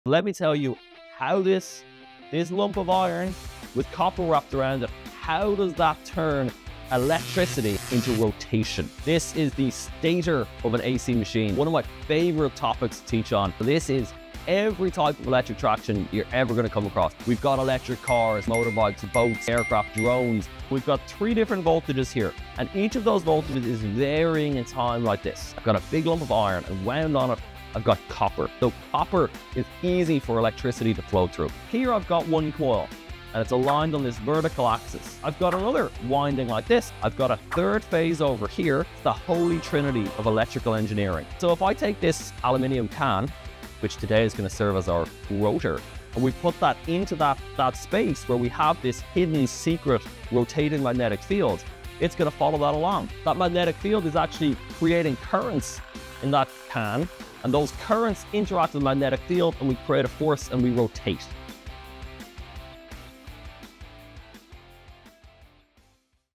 Learn How to Turn Electricity Into Rotation in 60 Seconds | 1 Minute Lectures | UCD Global
How does electricity turn into rotation? ⚡️➜🌀

Dr Paul Cuffe, assistant professor in the School of Electrical and Electronic Engineering, takes us on a quick dive of one of his favourite topics.

AC motors date back the 1880s and are having a bit if a reSURGEnce, if you will, with the recent improvements in batteries and semiconductors. 🔋